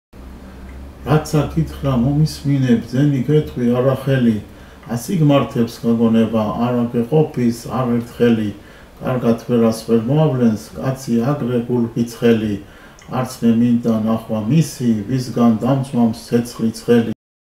SHOTA RUSTAVELI
SHALOM BAREL READS FROM THE GREAT POEM :
THE KNIGHT IN THE PANTER'S SKIN.